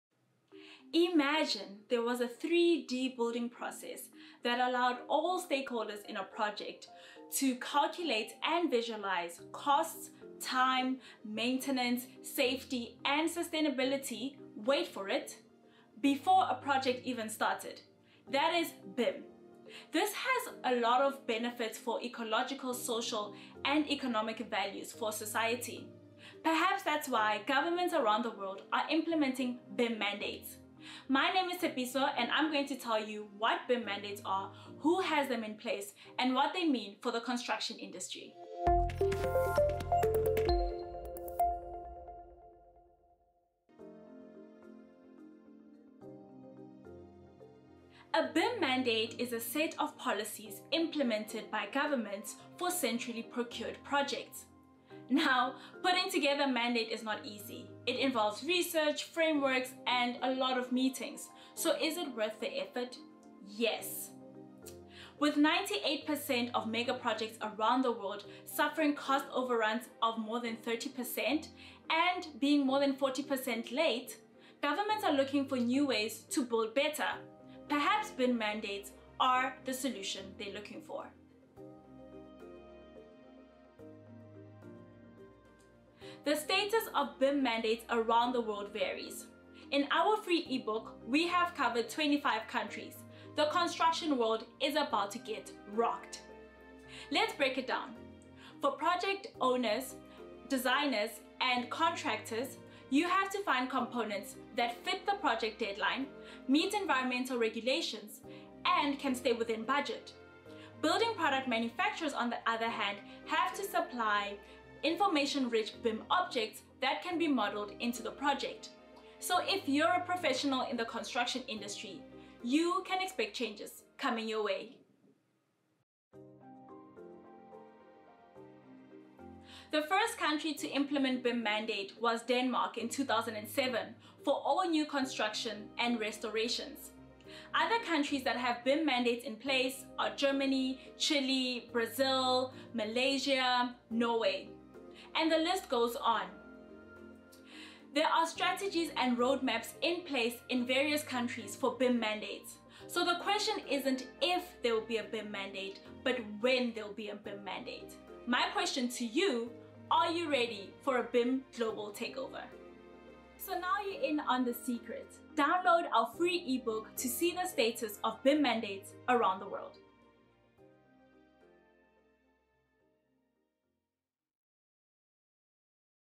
Global guide to BIM Mandates around the world
Governments are mandating BIM around the world.
Different countries have varying reasons for doing it, but it certainly shows us that the construction industry is changing- albeit slower than other industries.

Professionals in the construction industry should stay informed about the changes happening around them, as these will have an impact on their work.

Want to find BIM objects: bimobject.com
Find other BIM content here: business.bimobject.com